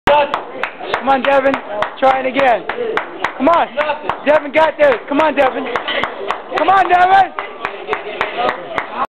My Friend Deven trying to do 225pounds but failed.